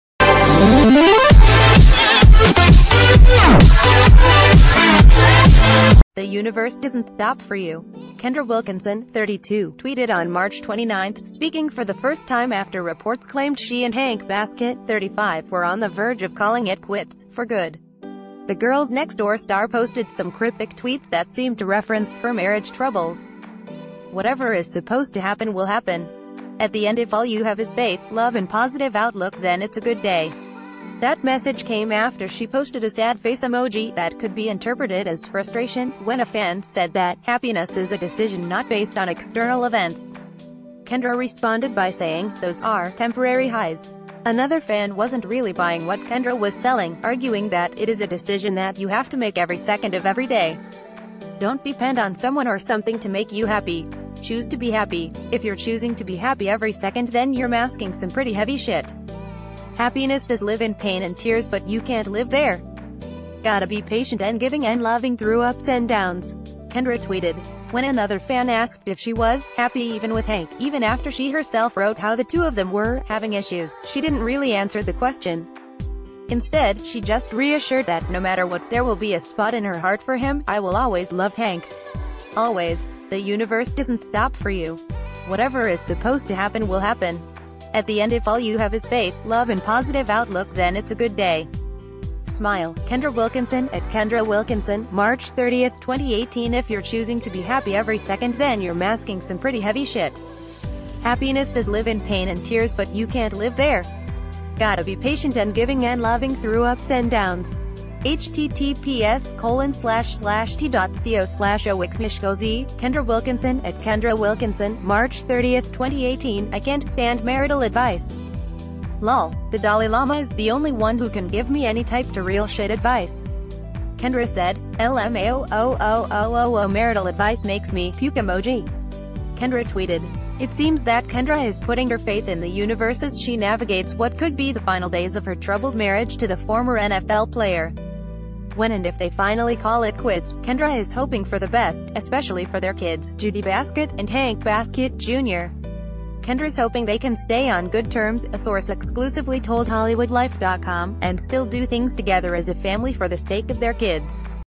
Kendra Wilkinson Speaks For 1st Time Since Divorce Rumors: What’s ‘Supposed To Happen Will Happen’
“The universe doesn’t stop for you,” Kendra Wilkinson, 32, tweeted on March 29, speaking for the first time after reports claimed she and Hank Baskett, 35, were on the verge of calling it quits – for ...